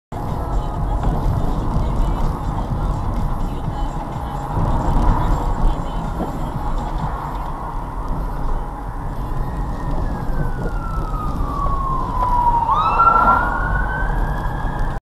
Police Scotland responding
PS BMW 330D roads policing seen on blues and twos up Lothian road